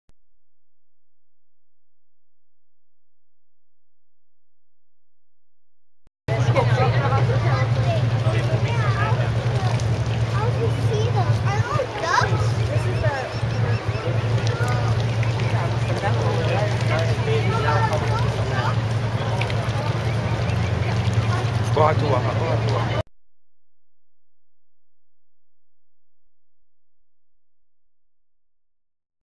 Swans and Swanlings or Cygnets
Aren't they sweet.  I found these in Strasbourg.